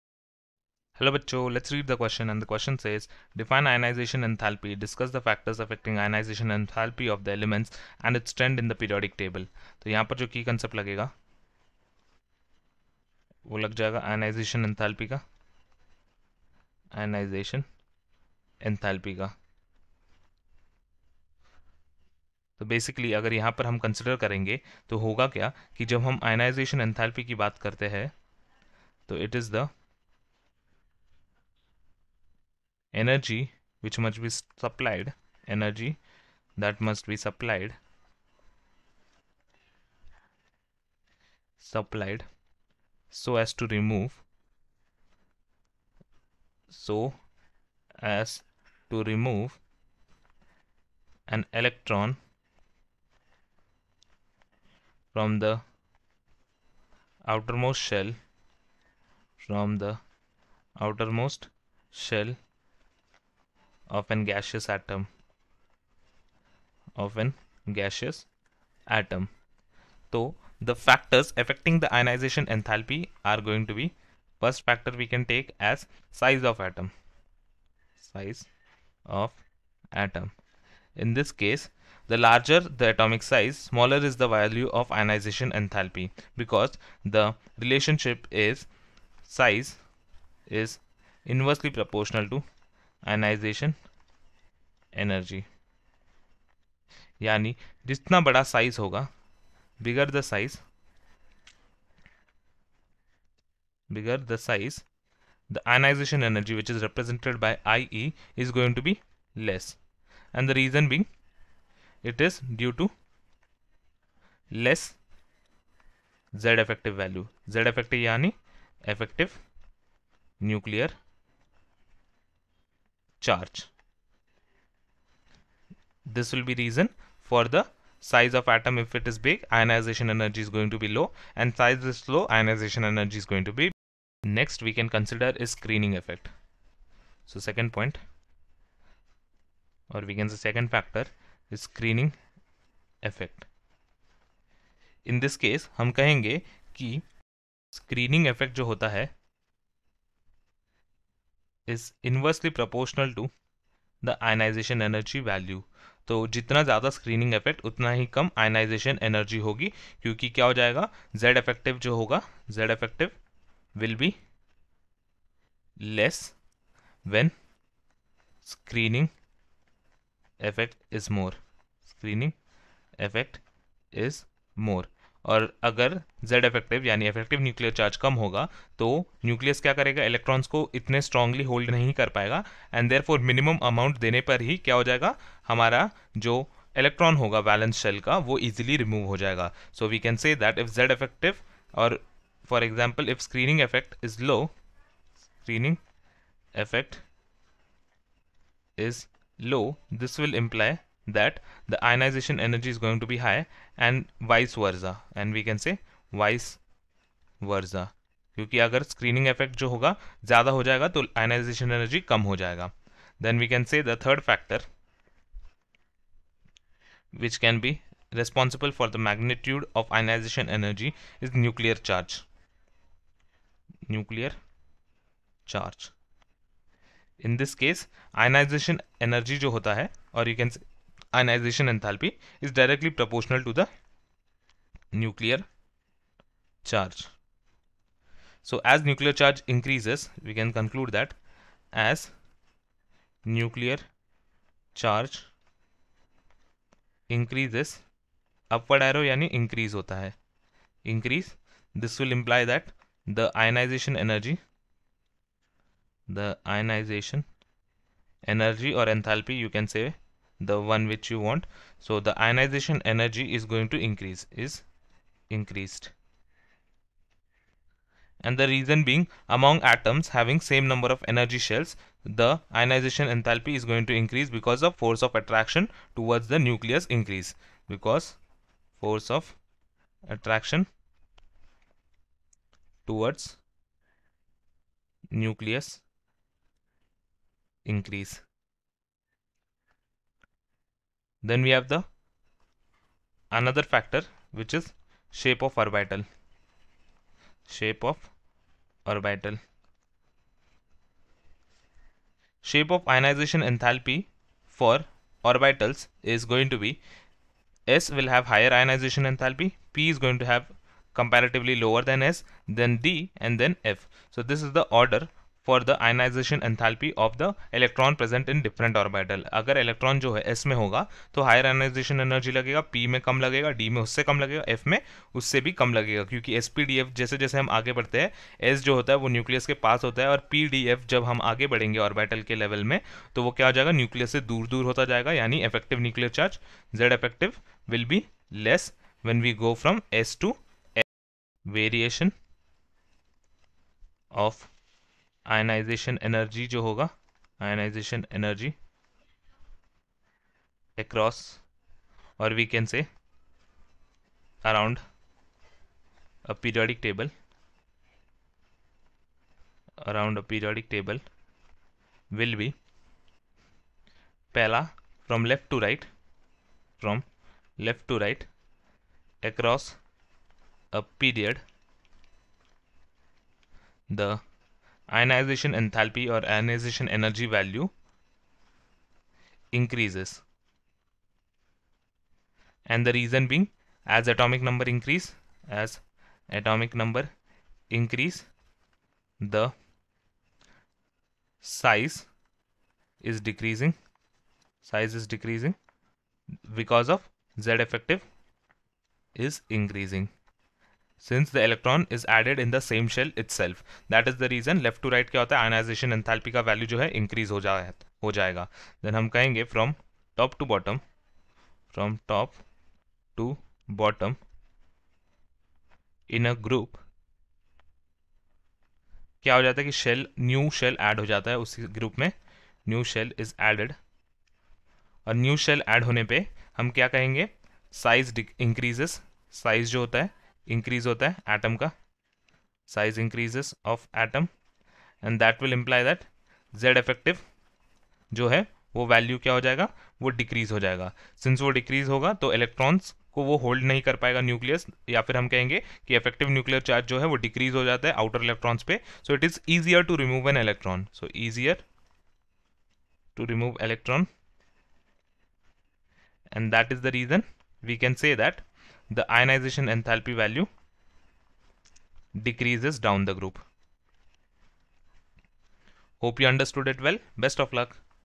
Define ionisation enthalpy. Discuss the factors affecting ionisation enthalpy of the elements and...
Define ionisation enthalpy. Discuss the factors affecting ionisation enthalpy of the elements and its trends in the periodic table.